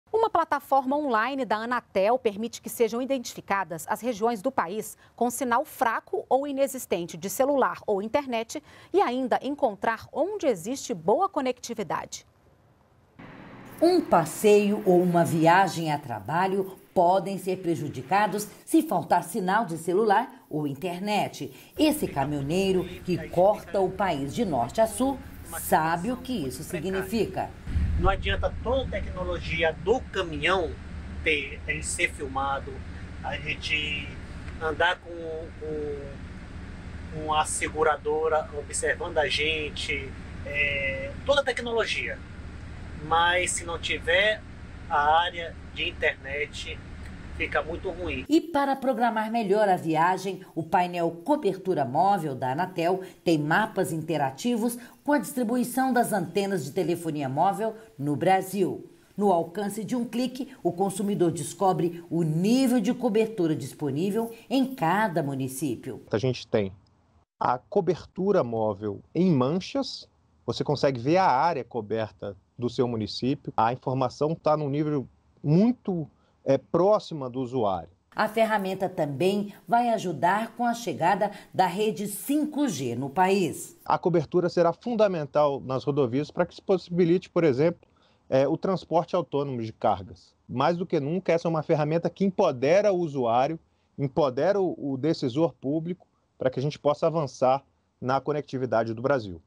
Qualidade sinal de internet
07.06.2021 – Uma plataforma online da Anatel permite que sejam identificadas as regiões do país com sinal fraco ou inexistente de celular ou internet e ainda encontrar onde existe boa conectividade.